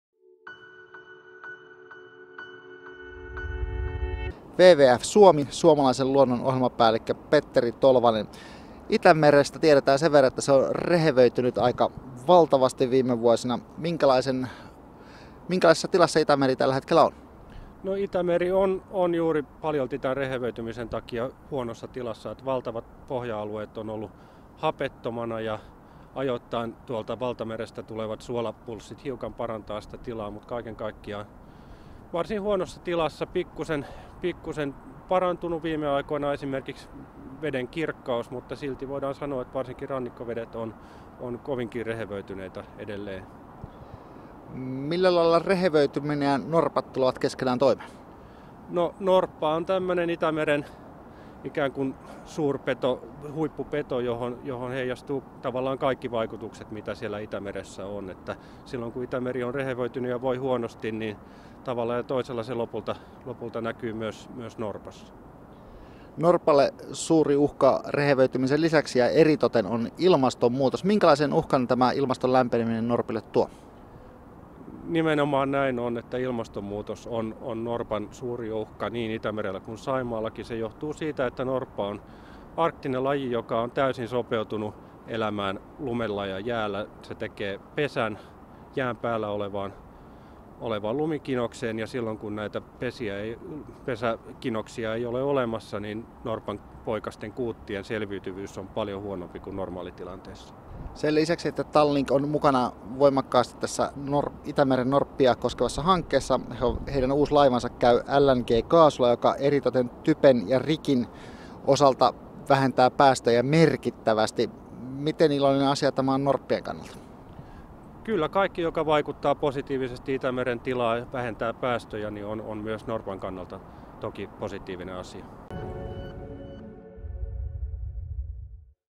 WWF Suomi Soome looduse projektijuht Petteri Tolvanen Läänemerest ja viigerhüljestest
Läänemere olukord on kohati murettekitav. Süvendite hapnikukadu ning eutrofeerumise tulemuseks olev sinivetikate õitsemine pakub kõneainet pea igal suvel. Kuidas mõjutab Läänemere olukord ohustatud Läänemere viigerhüljeste elu? Sellele teab vastust WWF Suomi Soome looduse projektijuht Petteri Tolvanen.